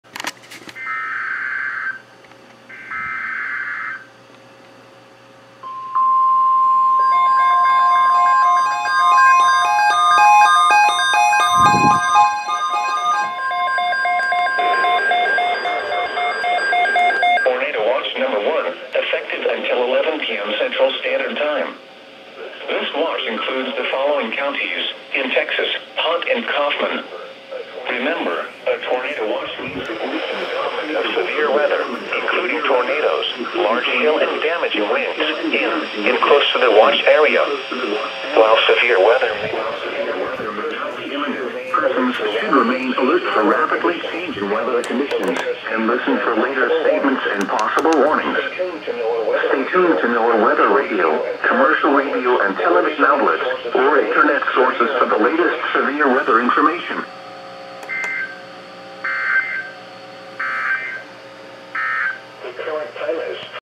First Watch of 2018! (Tornado Watch #1) (EAS #1650-1652)
Hard to believe this happening two years in a row.

Occurred on January 21, 2018.

KEC56, KHA99, WXK22